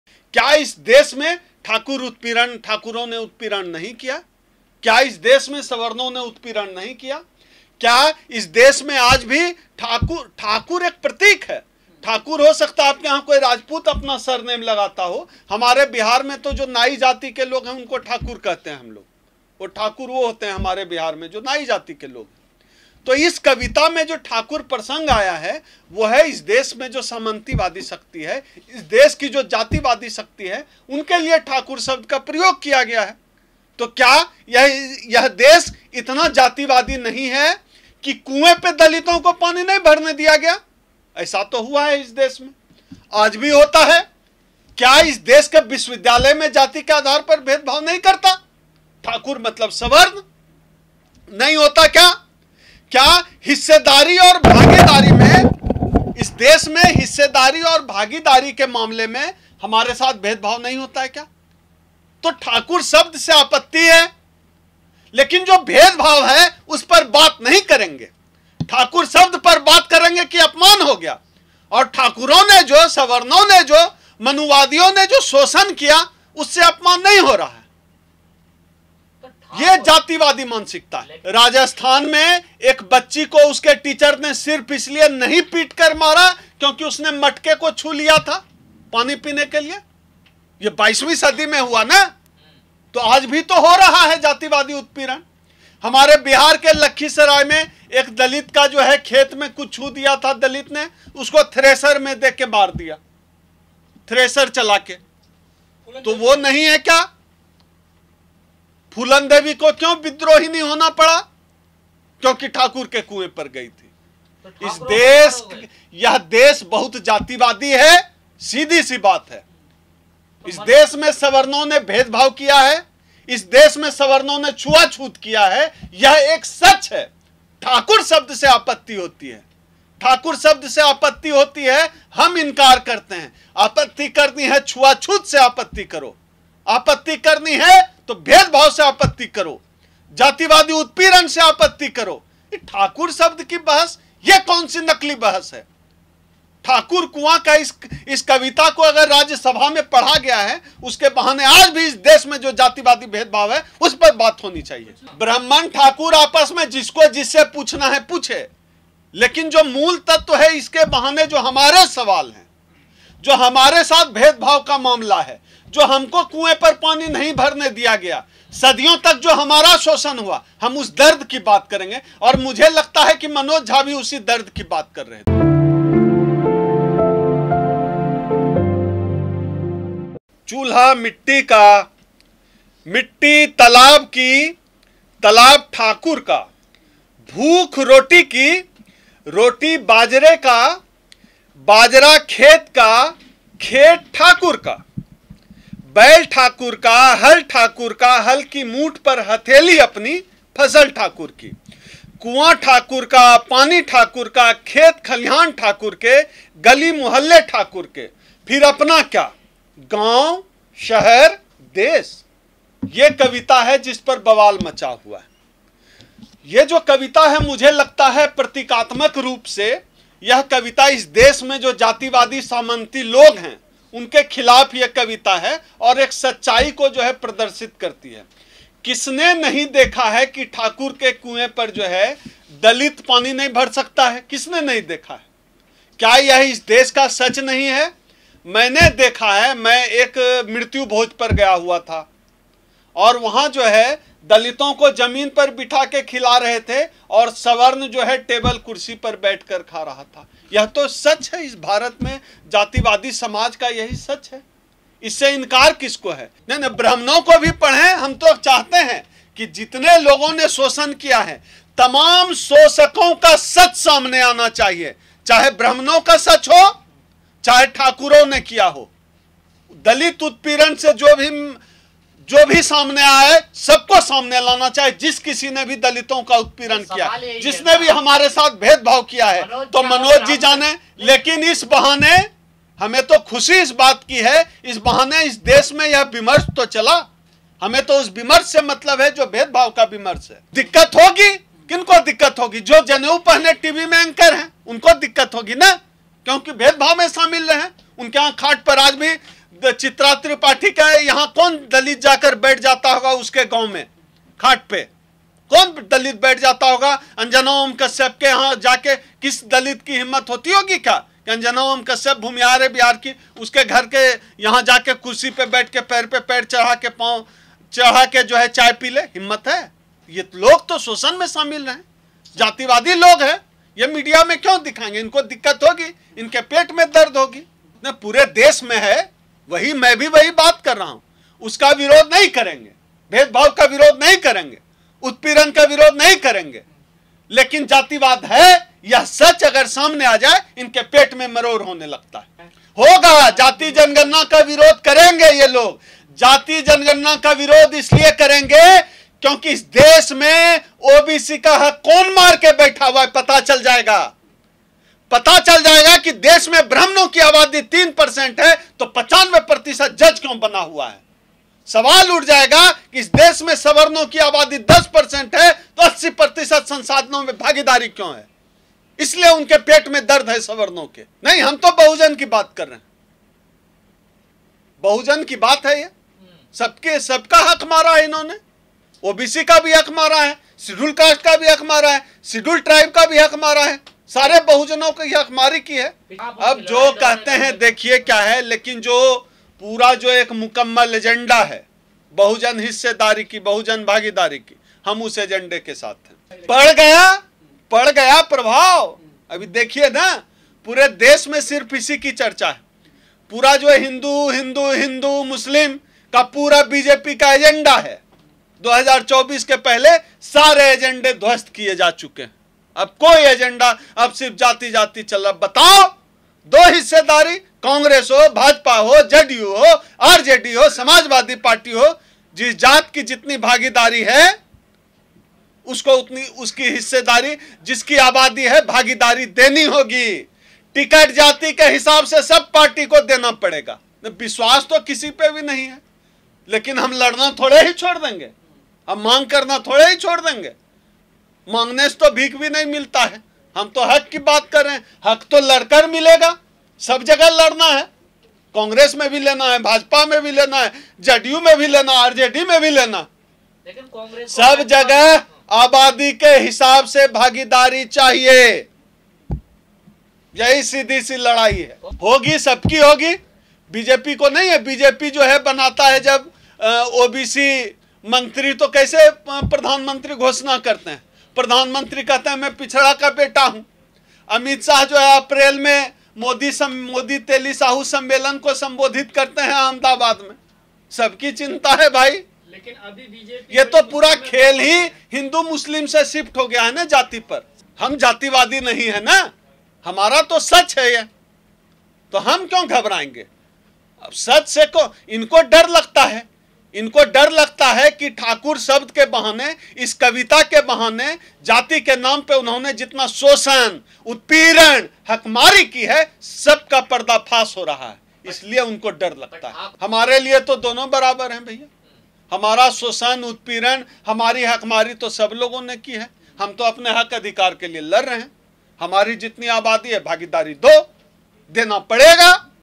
ठाकुर का कुंआ कविता से डरे मनुवादी, फूलनदेवी कर डाली थी सच्चा न्याय#thakurivlogs#omsudha#foolandevi
राज्यसभा में आरजेडी सदस्य मनोज झा ने ठाकुर का कुआ कविता सुनाई तो ठाकुर सरनेम वाले लोगों को दिक्कत हो गई। बिहार के नेता चेतन आनंद ने कहा कि मनोज झा हमको बदनाम कर रहे हैं। अखिल भारतीय परिसंघ के महासचिव डॉ ओमसुधा ने कहा कि ठाकुर का कुंआ कविता सुनकर ठाकुरों को अचंभा होता है लेकिन जातीय शोषण पर वह चुप रहते हैं।